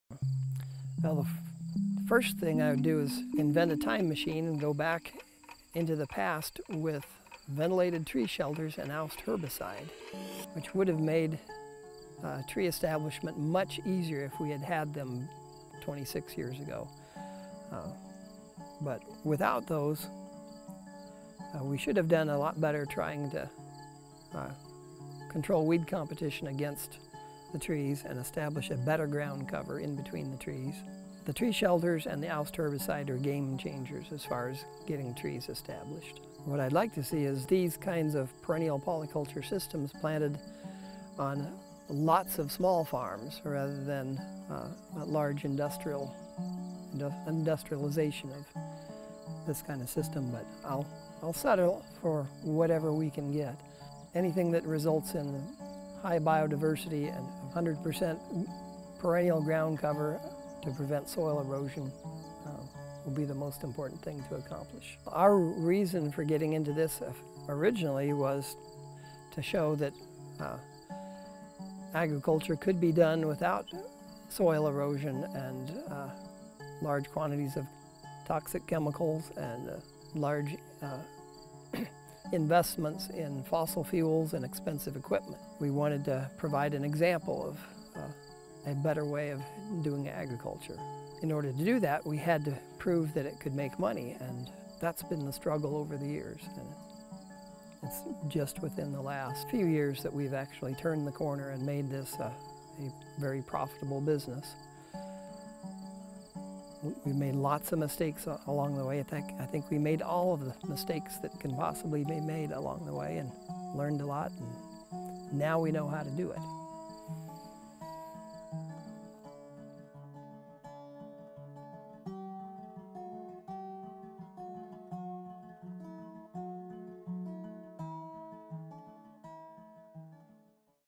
If Tom Wahl of Red Fern Farm had a Time Machine
If Tom Wahl of Red Fern Farm had a time machine, he just might do things differently. Lucky for us, we can learn from his mistakes and consider how we choose to move forward with establishing a perennial future.